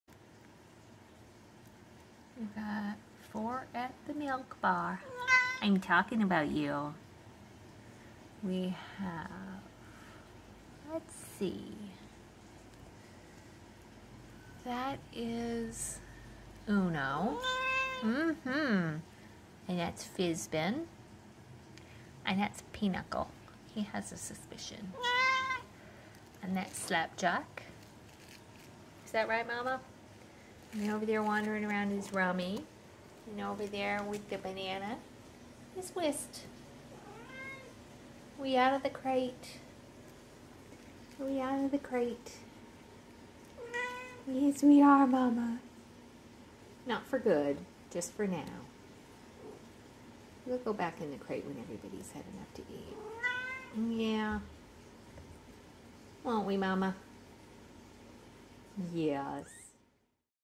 Kittens at the milk bar
They're 3 1/2 weeks old - usually kittens become interested in solid food around 5-6 weeks (some sooner, some later - depends on the kitten). Lucky for these guys, the milk bar is available nearly 24/7.